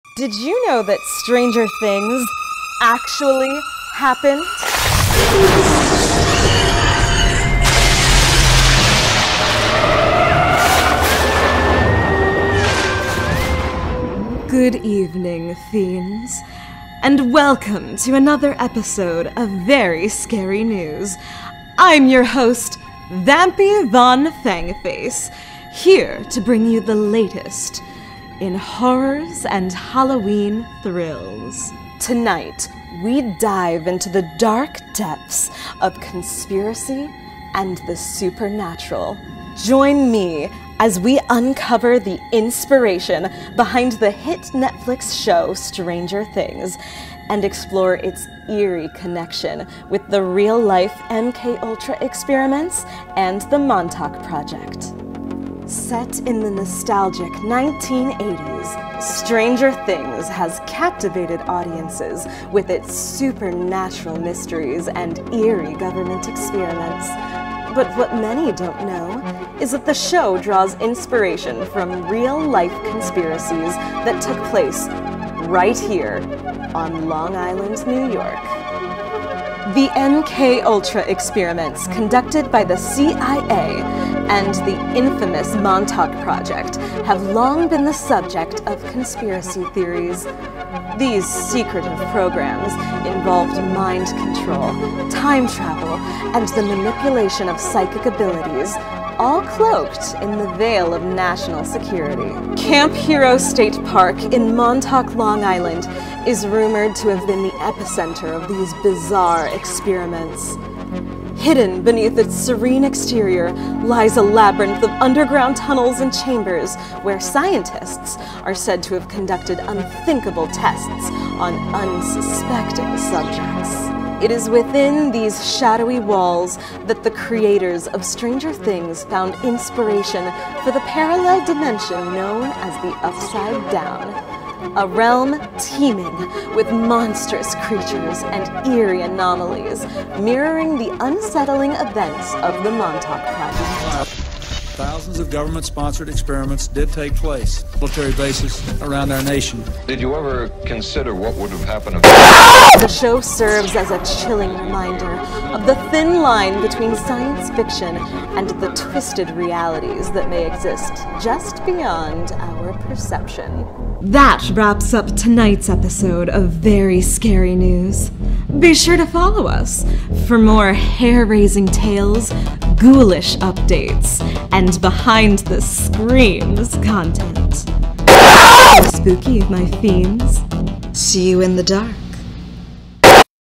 Where Stranger Things ACTUALLY HAPPENED? | Very Scary News | Season 1 : Episode 1
Prepare to witness an absolutely BONE-CHILLING new Horror News series, bringing you exclusive news regarding this year’s most anticipated Horror Movies, Haunted Houses, and all things Halloween - with your host, Vampy Von Fangface! 🎃

The first episode in Season 1 covers the hit Netflix show Stranger Things - and where the real-life government experiments that inspired the show occurred! Watch and find out how an unassuming military-base-turned-state-park in Montauk, Long Island played host to some of history's worst crimes against humanity.